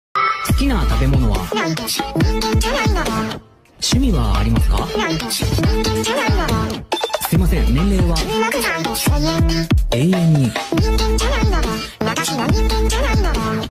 Im not human • DDLC • Gacha club
this is something I feel like I would make a year ago, also when I heard this audio I thought of Monika.